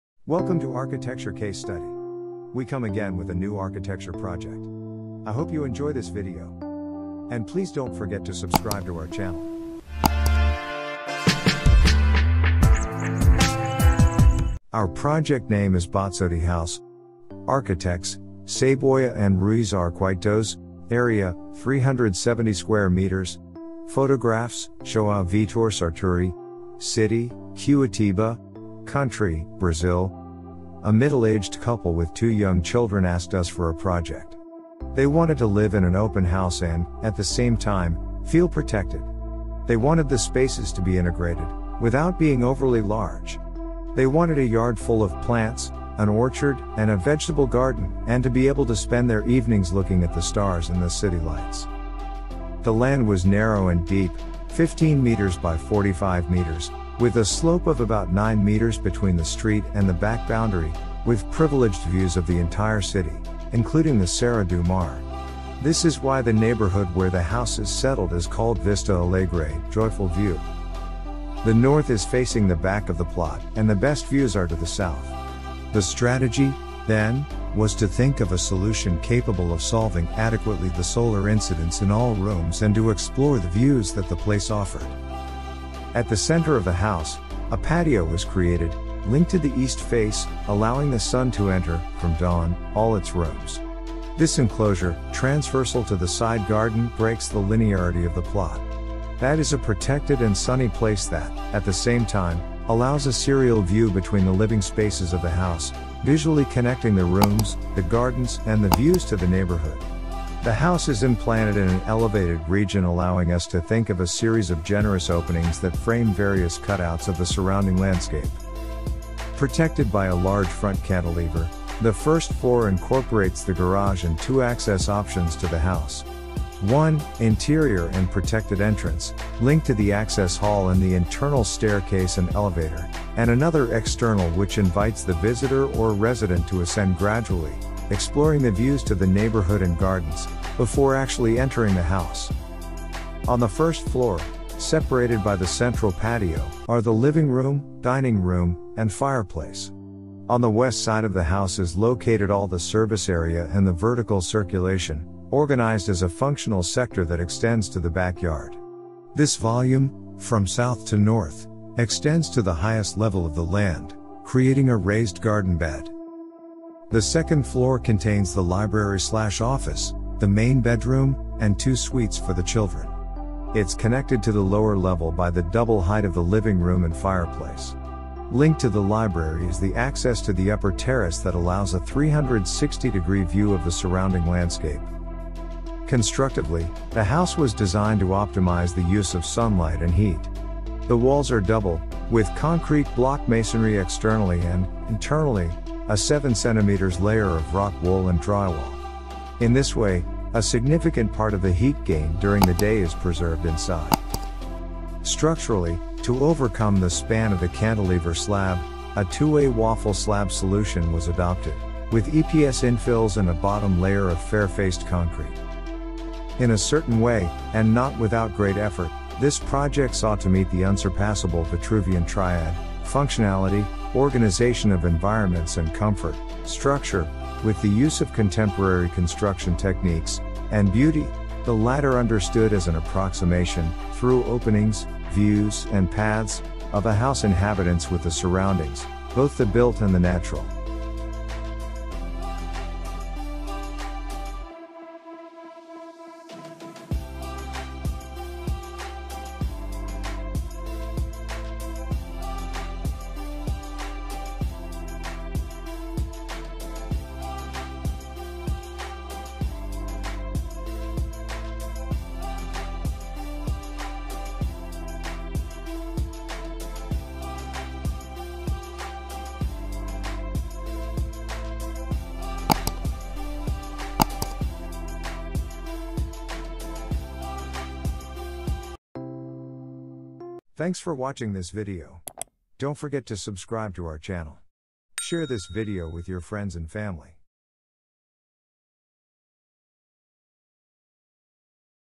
Bazzotti House | Saboia + Ruiz Arquitetos | Curitiba, Brazil
Architects: Saboia+Ruiz Arquitetos
Area:  370 m²
Year:  2021
Photographs:  João Vitor Sarturi

    Manufacturers: Alucon, Alucon Esquadrias (Linha Infinite S32), Carlos Alberto de Paula, Coifatec, Hidroforma, Nogueira Iluminação, Pau e água, Portobello, Scrock pisos

    Author: Alexandre Ruiz
    Collaborators:Bruno Bueno Niepsui, Rodrigo Philippi Vinci e João Vitor Sarturi
    Concrete Structure: Mauer Egas
    Site Area:630m²
    City: Curitiba
    Country: Brazil

(N.B. ALL PICTURES AND INFORMATION COLLECTED FROM INTERNET)